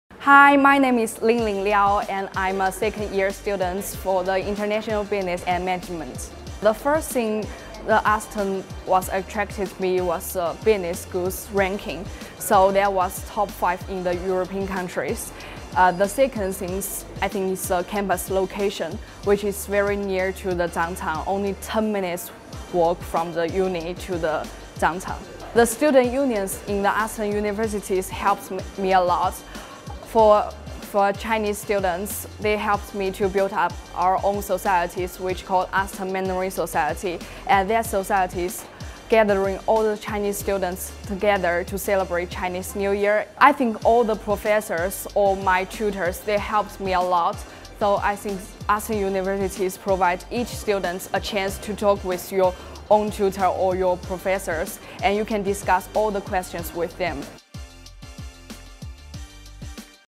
HERE IS INTERNATIONAL BUSINESS & MANAGEMENT
Linglin takes you through her experience of studying International Business & Management as an international student.

You can find more detail, including international fees, courses, visas etc